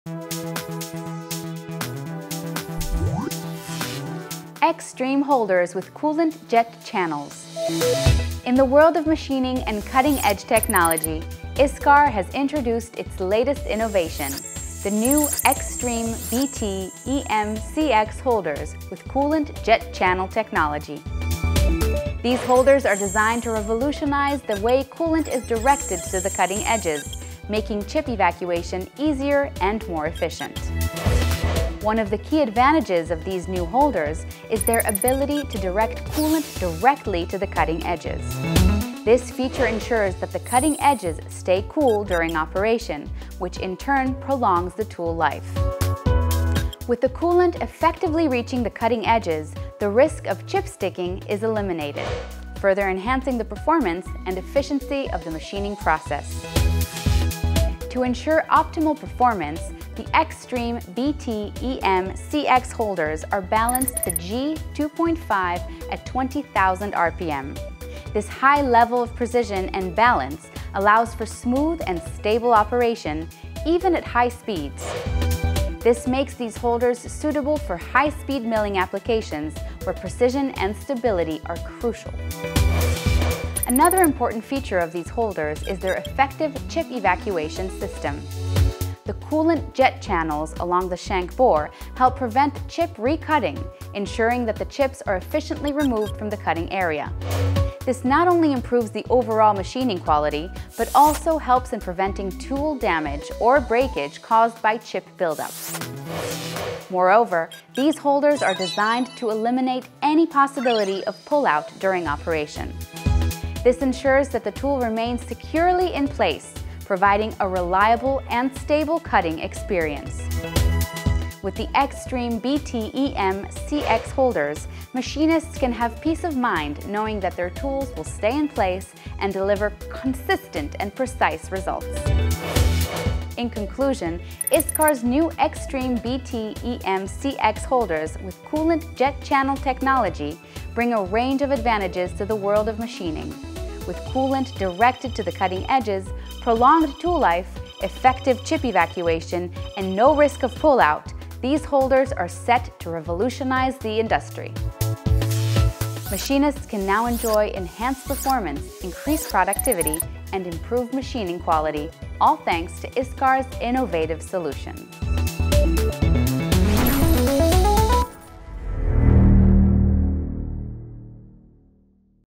ISCAR TECH TALK - X-STREAM Holders with Coolant Jet Channels
In the world of machining and cutting-edge technology, ISCAR has introduced its latest innovation: the new X-STREAM BT EM-CX holders with Coolant Jet Channel technology. These holders are designed to revolutionize the way coolant is directed to the cutting edges, making chip evacuation easier and more efficient.

One of the key advantages of these new holders is their ability to direct coolant directly to the cutting edges. This feature ensures that the cutting edges stay cool during operation, which in turn prolongs the tool life. With the coolant effectively reaching the cutting edges, the risk of chip sticking is eliminated, further enhancing the performance and efficiency of the machining process.

To ensure optimal performance, the X-STREAM BT EM-CX holders are balanced to G2.5 at 20,000 RPM. This high level of precision and balance allows for smooth and stable operation, even at high speeds. This makes these holders suitable for high-speed milling applications, where precision and stability are crucial.

Another important feature of these holders is their effective chip evacuation system. The coolant jet channels along the shank bore help prevent chip re-cutting, ensuring that the chips are efficiently removed from the cutting area. This not only improves the overall machining quality but also helps in preventing tool damage or breakage caused by chip buildup.

Moreover, these holders are designed to eliminate any possibility of pull out during operation. This ensures that the tool remains securely in place, providing a reliable and stable cutting experience. With the X-STREAM BT EM-CX holders, machinists can have peace of mind knowing that their tools will stay in place and deliver consistent and precise results.

In conclusion, ISCAR's new X-STREAM BT EM-CX holders with Coolant Jet Channel technology bring a range of advantages to the world of machining. With coolant directed to the cutting edges, prolonged tool life, effective chip evacuation, and no risk of pull out, these holders are set to revolutionize the industry. Machinists can now enjoy enhanced performance, increased productivity, and improved machining quality, all thanks to ISCAR's innovative solution.

Download ISCAR WORLD, the virtual "one stop shop" app that features all ISCAR's online apps, interfaces, and product catalogs in a single space: